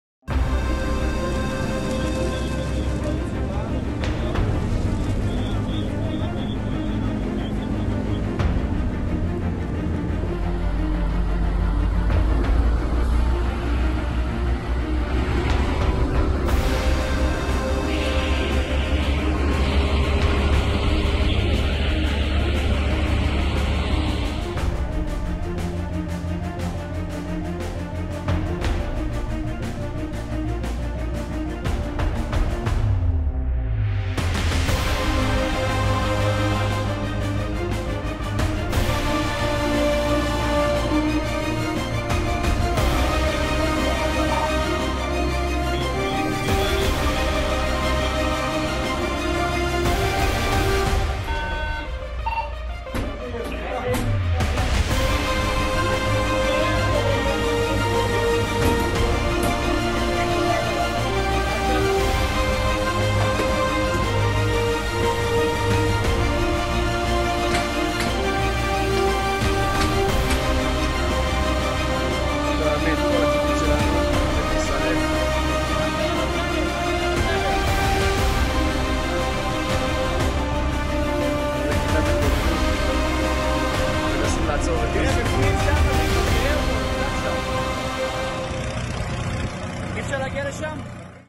امتدت لمناطق شاسعة وأصابت 6.. اندلاع حرائق غابات في إسرائيل جراء صواريخ حزب الله
امتدت لمناطق شاسعة بينها قواعد عسكرية وتسببت في إصابة 6 بين فرق الإطفاء.. اندلاع حرائق غابات في شمال إسرائيل جراء صواريخ أطلقتها جماعة حزب_الله من

لمعرفة أحدث المستجدات.. عبر البث المباشر من هنا ⤵